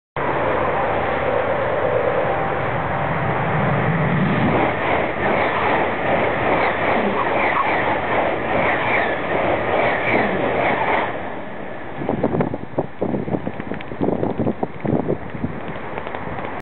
Train dodging
Me in Germany, outside of Frankfurt with the ICE trains